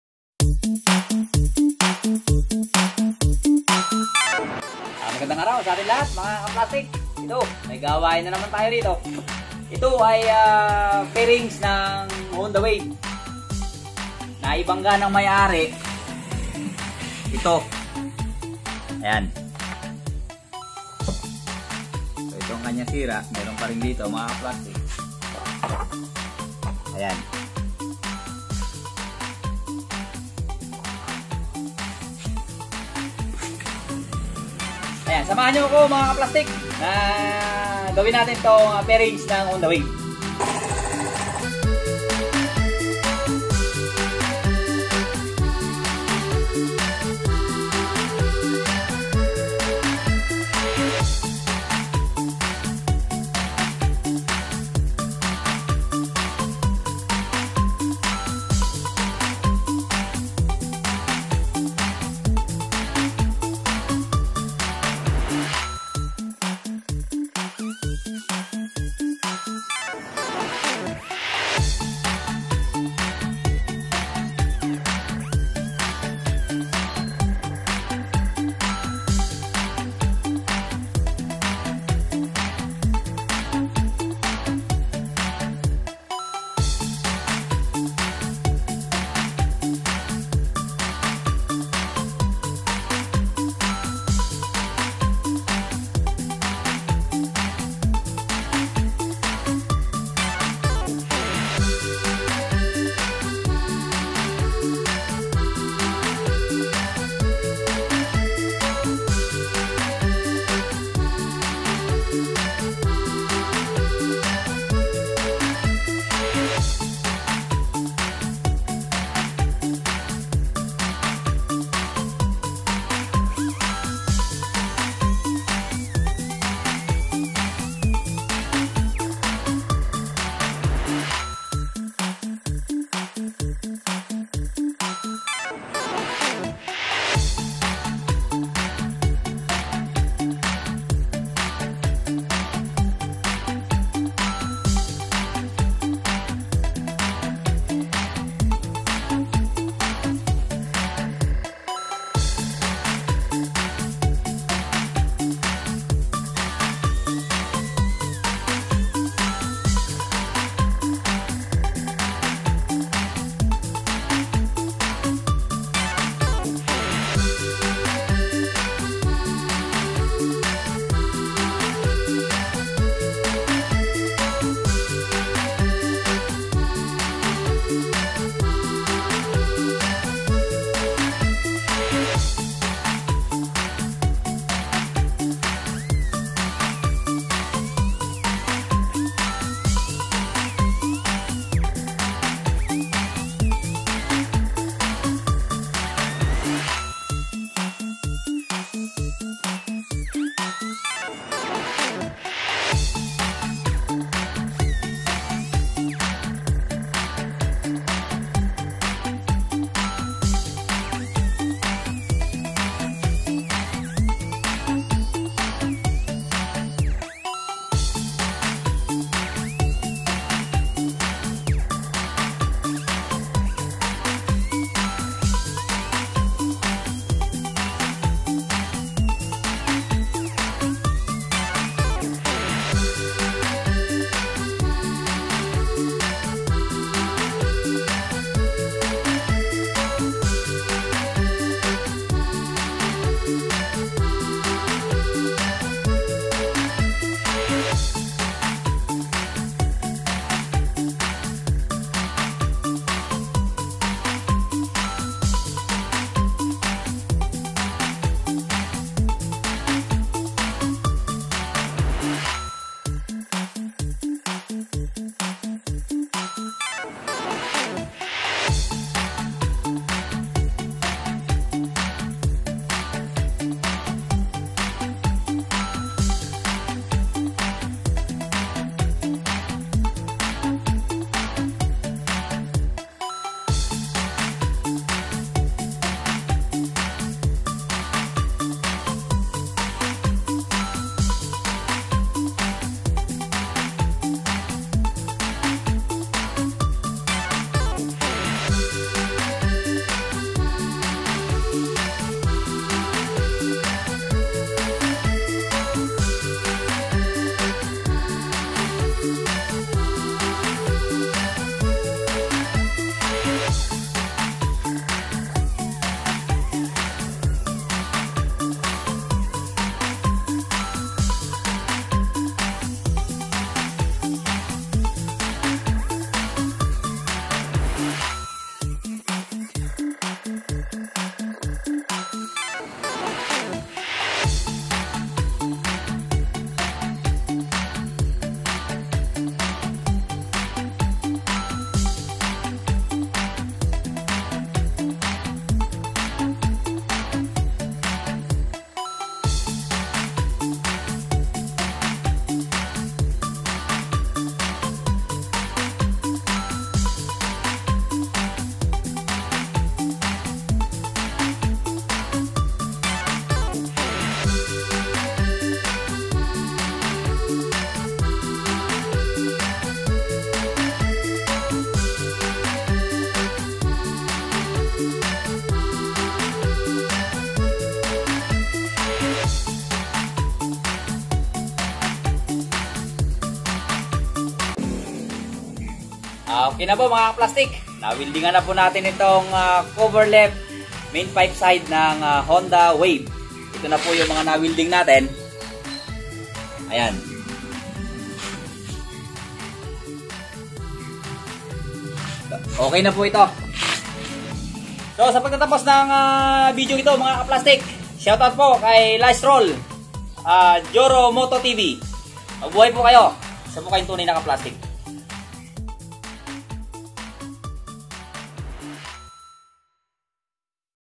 plastic welding ng basag na MAIN PIPE SIDE COVER ng HONDA WAVE |P AND P PMOTORWORKZ
plastic welding ng basag na MAIN PIPE SIDE COVER ng HONDA WAVE

YOUTUBE CHANNEL: P AND P MOTORWORKZ